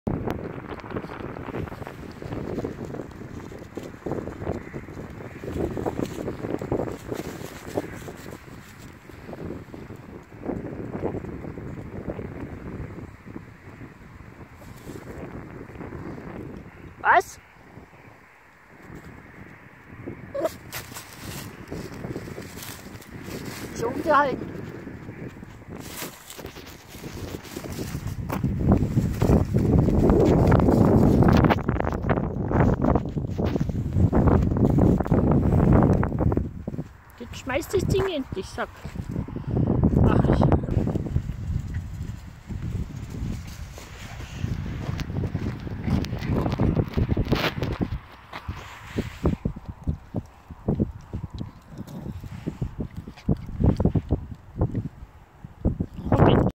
Nelly die Fluse
Frauchen animieren zum werfen , ist garnicht einfach die hat nur filmen im Kopf